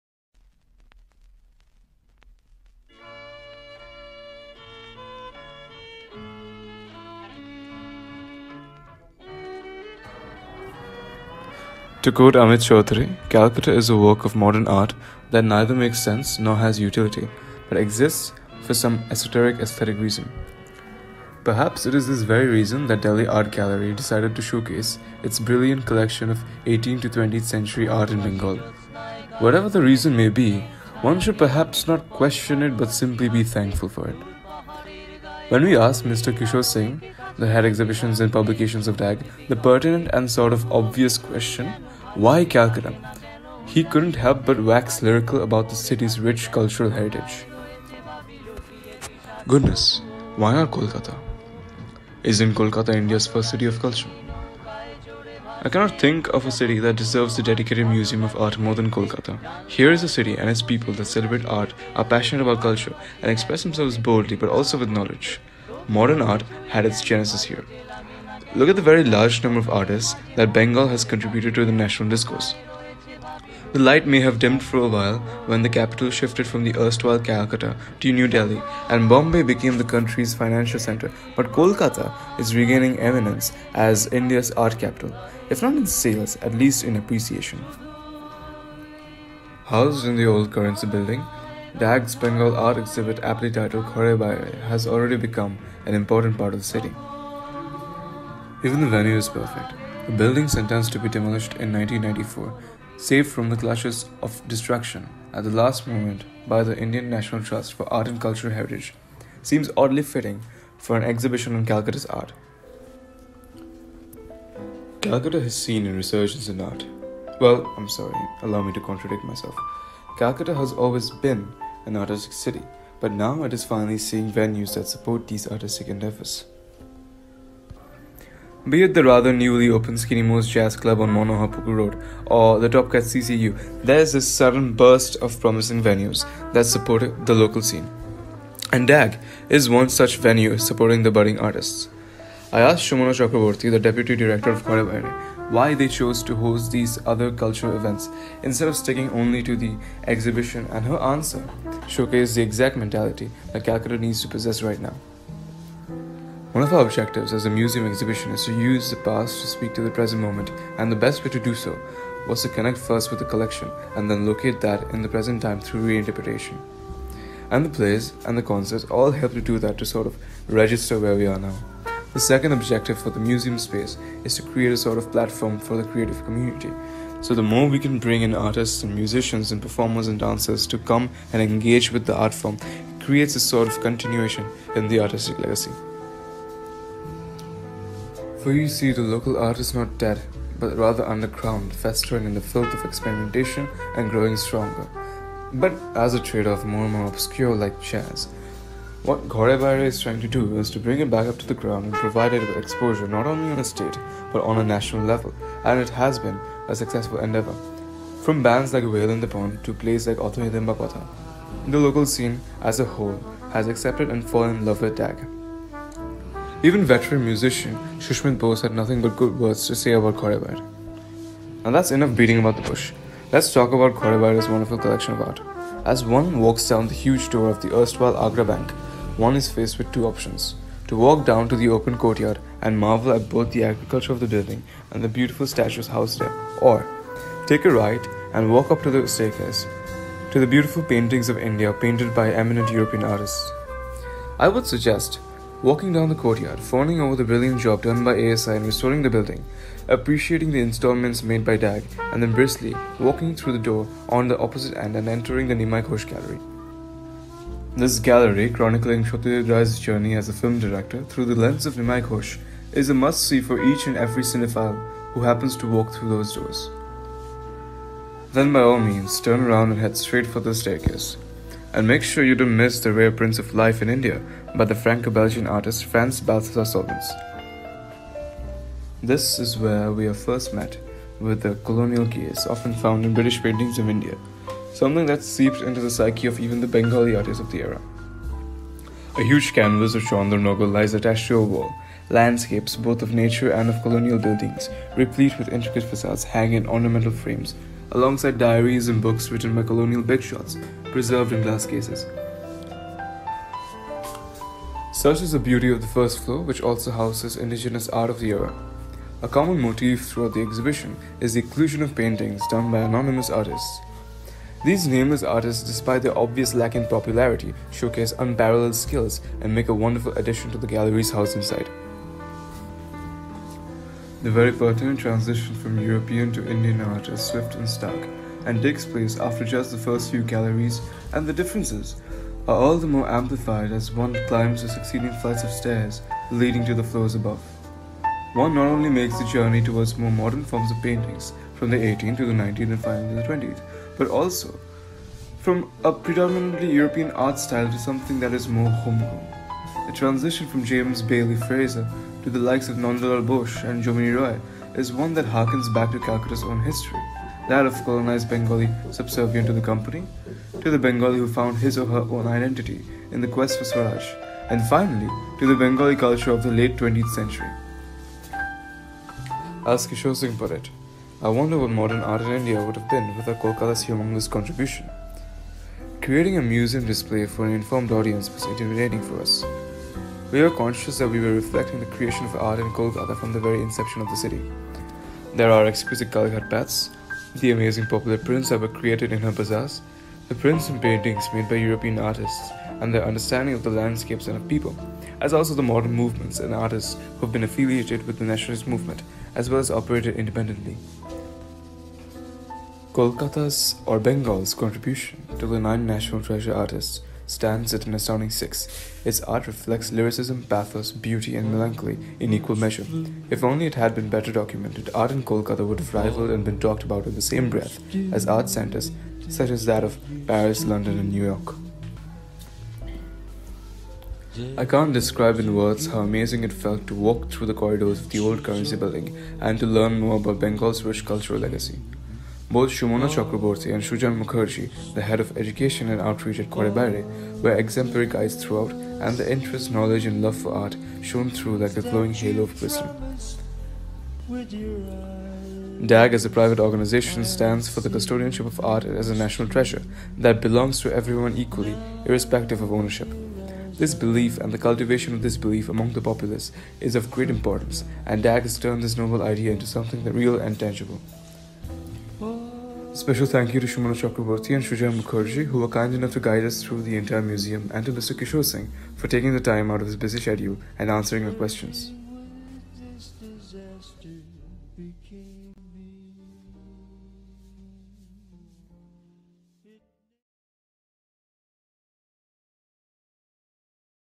Ghare Baire: A Cultural Juggernaut
On the 7th of February, the Monograph team visited the Ghare Baire exhibit housed in the Old Currency Building.

Sumona Chakravarty (Deputy Director, Ghare Baire, DAG) and Sujaan Mukherjee (Education and Outreach) were kind enough to give us a tour of the gallery and they also put us in touch with Mr. Kishore Singh (Head of Exhibitions and Publications, DAG).

This article would not have been possible without them

Video Shot & Edited by Aryaman Manna
Narrated by Ayush Chakraborty
Written by Anuraag Das Sarma & Indrayani Bhadra